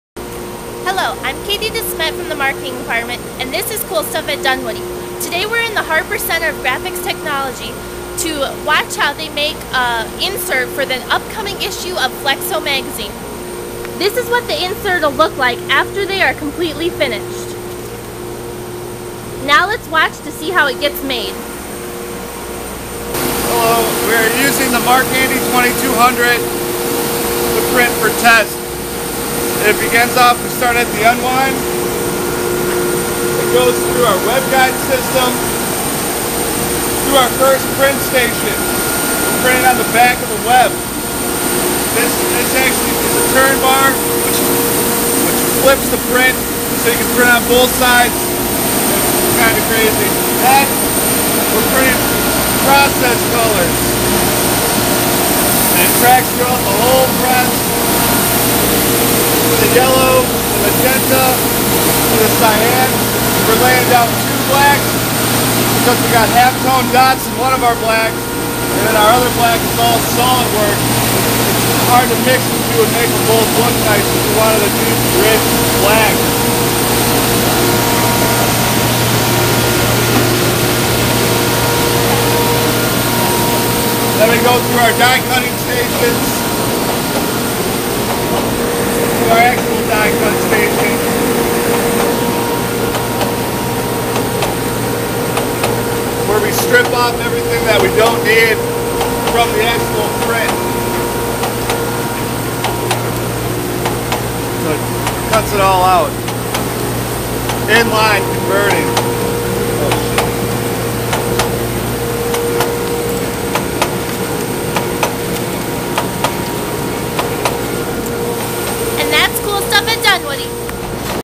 Cool Stuff at Dunwoody: Flexographic Printing
Cool Stuff at Dunwoody visits the Harper Center of Graphics Technology  for a demonstration of the Mark Andy 2200H Flexographic Inline Narrow web press.